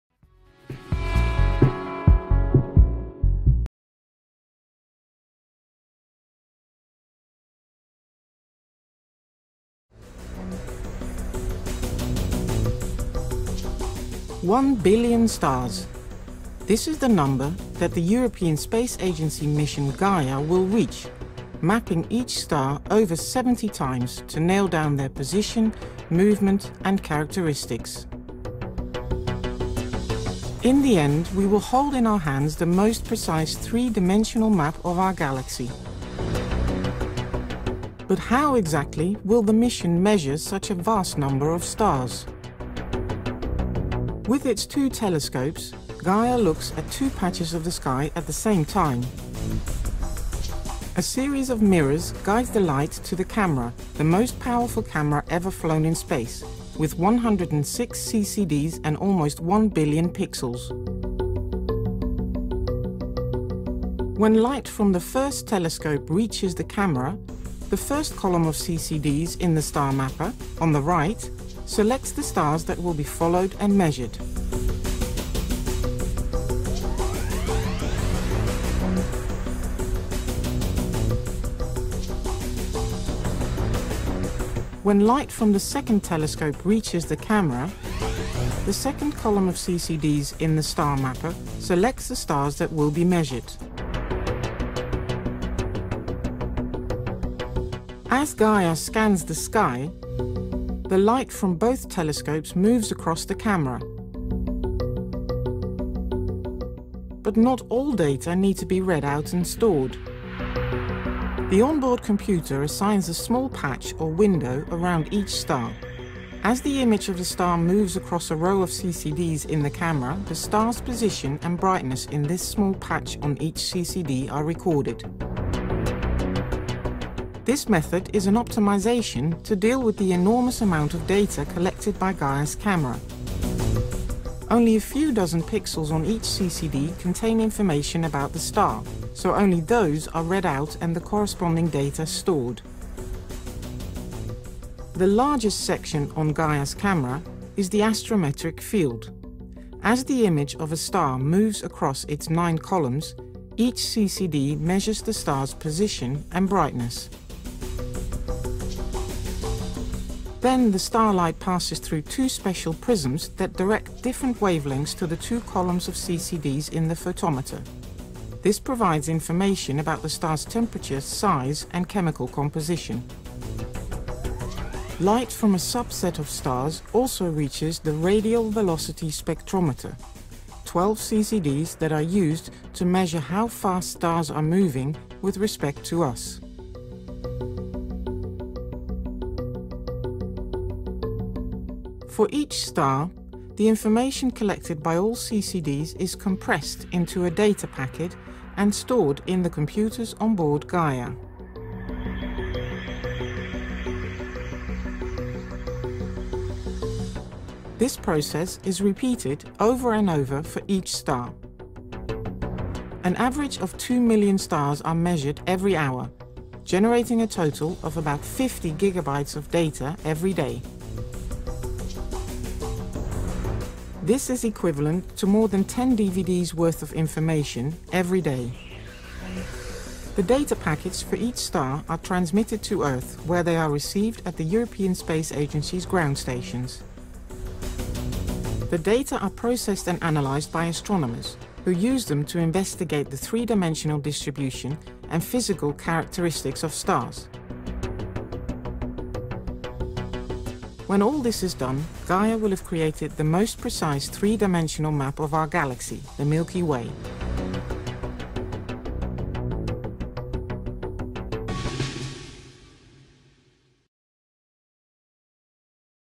Most Powerful Camera To Fly In Space To Snap 3D Milky Way Map | Video
ESA's Gaia mission's billion pixel camera will create the most accurate three dimensional map of our galaxy (mapping a billion stars). Find out how it will do it. The mission is scheduled to launch on December 19th, 2013.